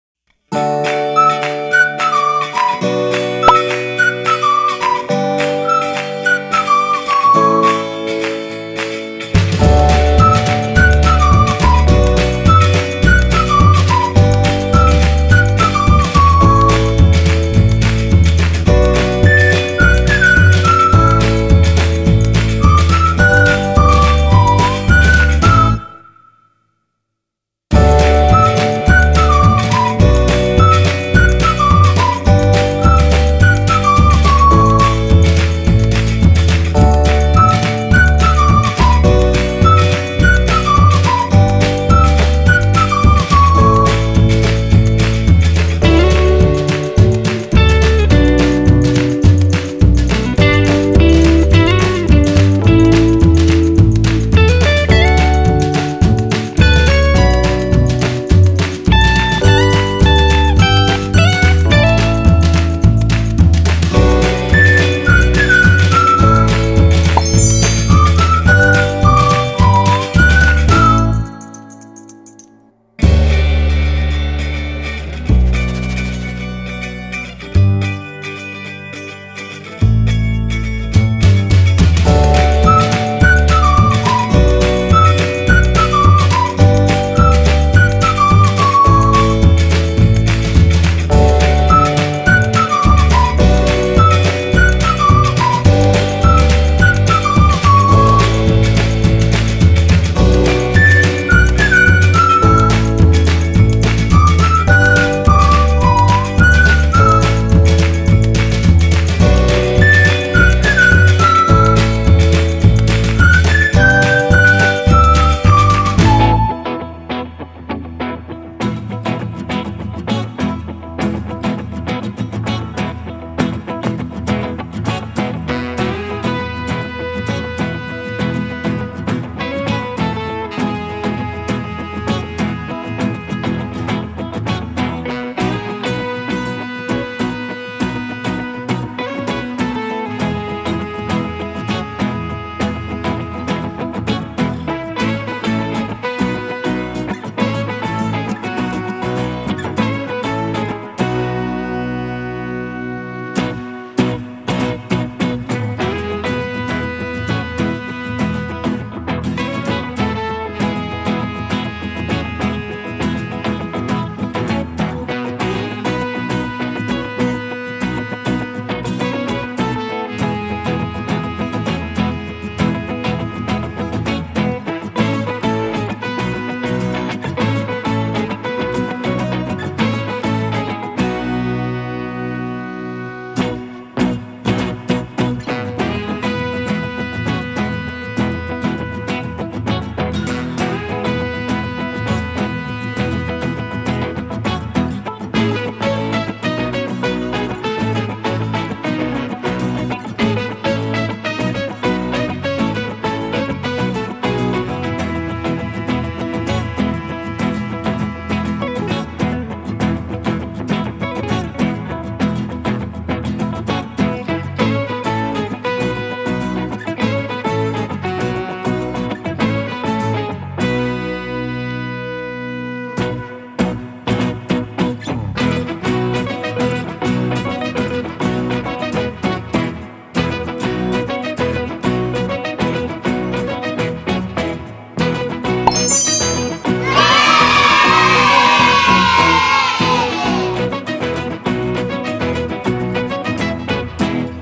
Drawing and Painting Ice cream for Kids & Toddlers | Simple Art Tip
how to draw ice cream for kids with Colored Marker. Drawing and Painting Ice cream for Kids & Toddlers | Simple Art Tip
ice cream coloring page for kids